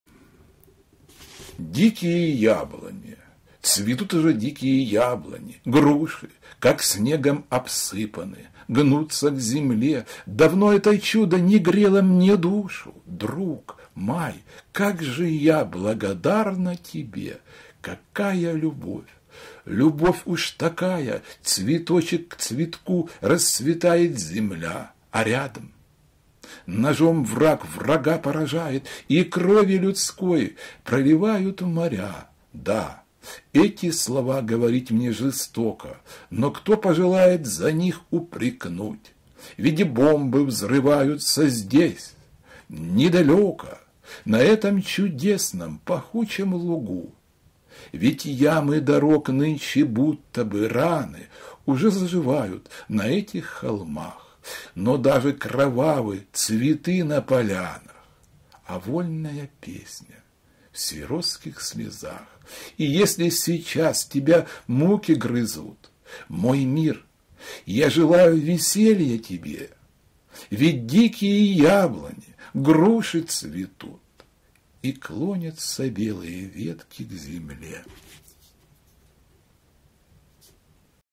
Дикие яблоки , Саломея Нерис, читает Павел Беседин
Цветут уже дикие яблони, груши,
Как снегом обсыпаны, гнутся к земле.
Давно это чудо не грело мне душу.
Друг, май, как же я благодарна тебе!

Какая любовь! Любовь уж такая-
Цветочек к цветку расцветает земля.
А рядом – ножом враг врага поражает
И крови людской проливают моря.

Да, эти слова говорить мне жестоко,
Но кто пожелает за них упрекнуть?
Ведь бомбы взрываются здесь, недалеко-
На этом чудесном пахучем лугу.

Ведь ямы дорог нынче будто бы раны,
Уже заживают на этих холмах.
Но даже кровавы цветы на полянах,
А вольная песня- в сиротских слезах.

И если сейчас тебя муки грызут,
Мой мир, я желаю веселья тебе-
Ведь дикие яблони, груши цветут
И клонятся белые ветки к земле.